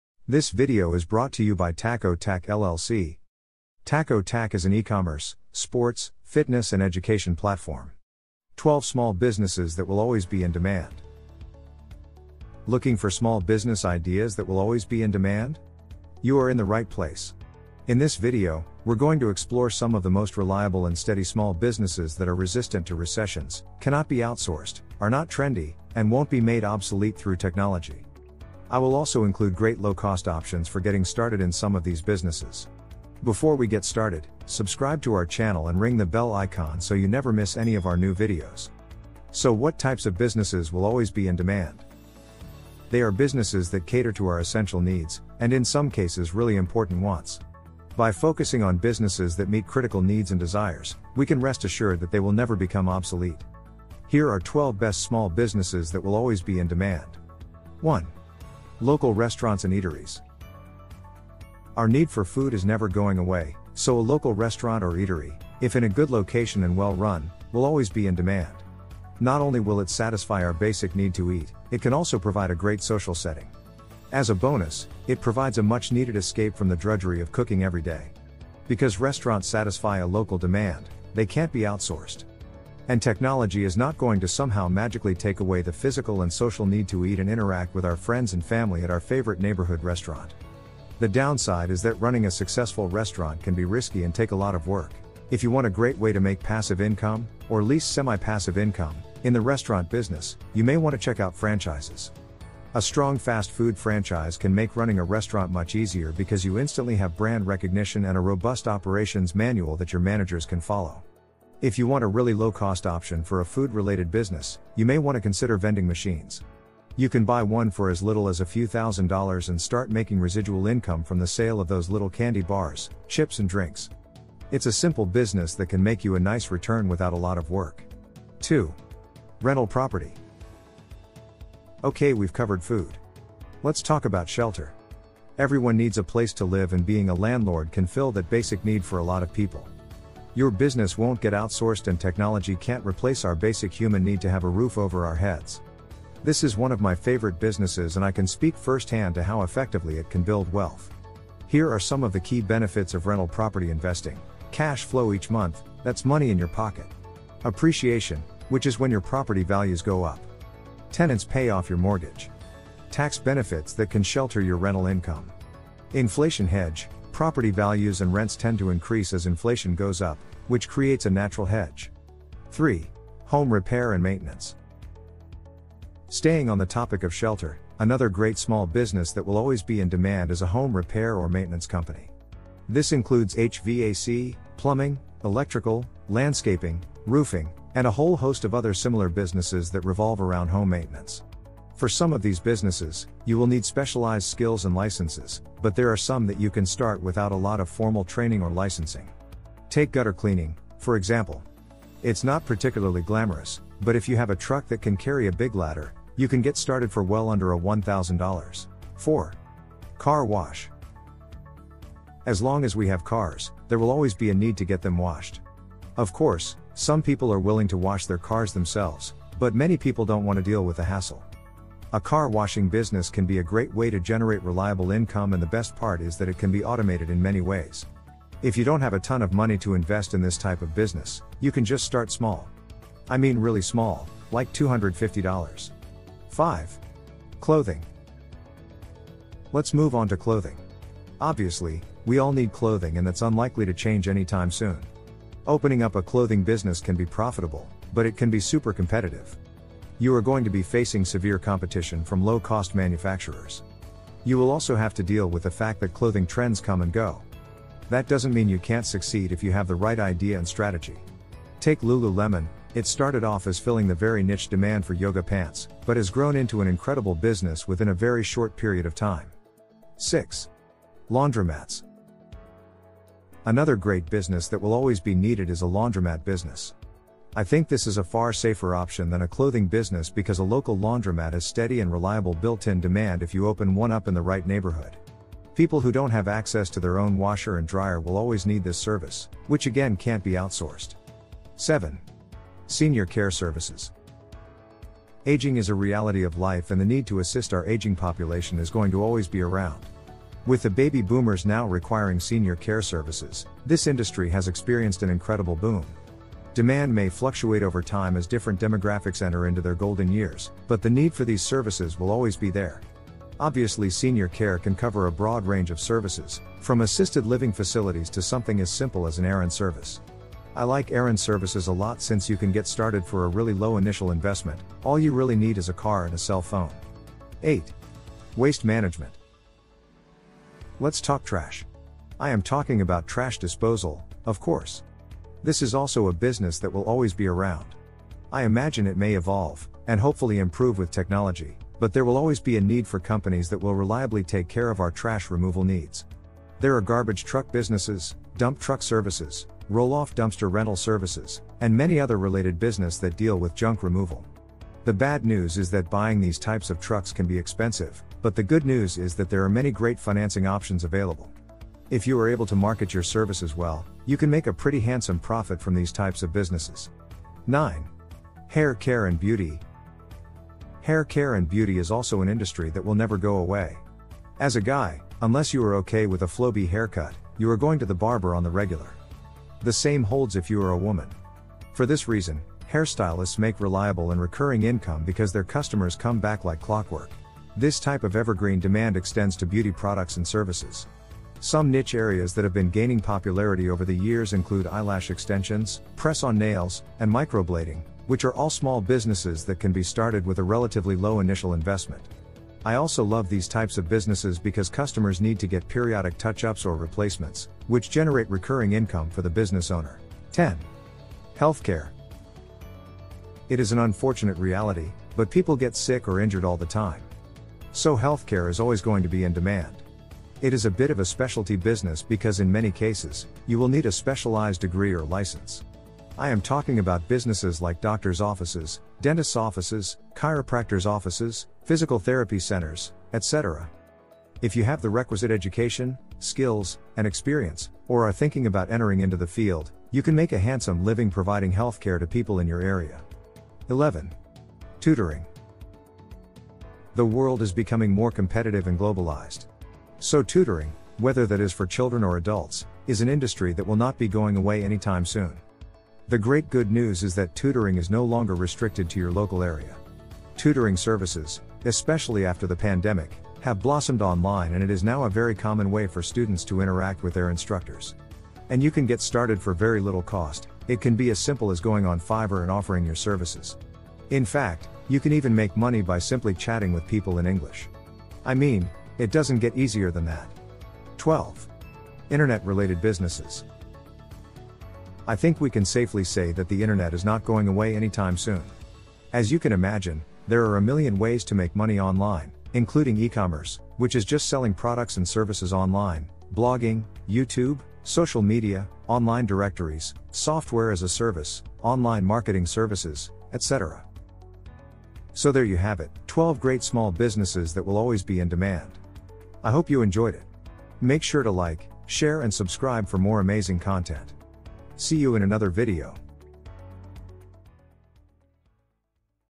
12 Business Ideas That Will Always Be In Demand
If you have always wanted to start a business, but don't have the money to begin one,
you are in the right place. In this video, I am going to share 12 businesses that will always be in demand.

What's even better is that all of them can be started on the side and in your spare time.

In other words, literally anyone can start these businesses if they have the determination to do it. Keep watching to know more about them.